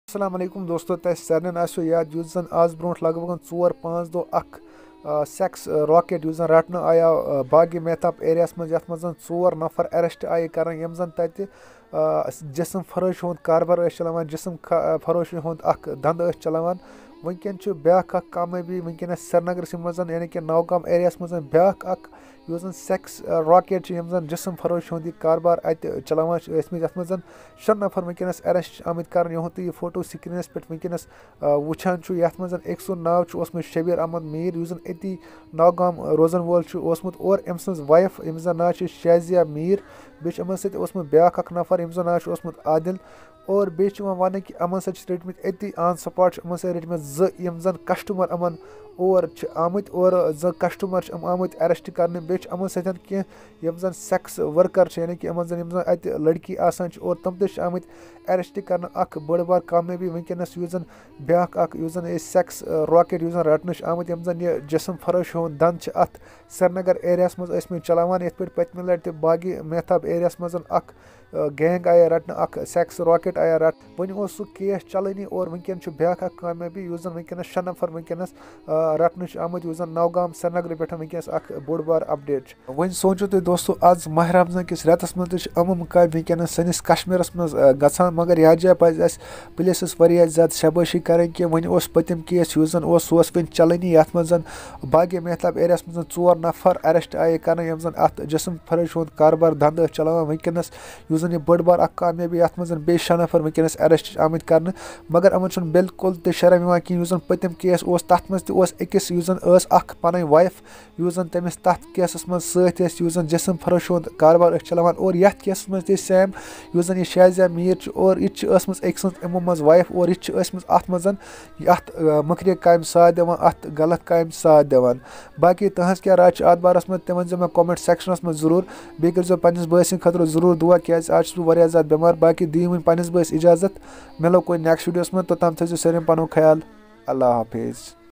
Aur Ak Sèx Rackèt Bustèd 2 Fèmale Sèx wórker From Srìnàgár 😭
Kashmiri drama
kashmiri songs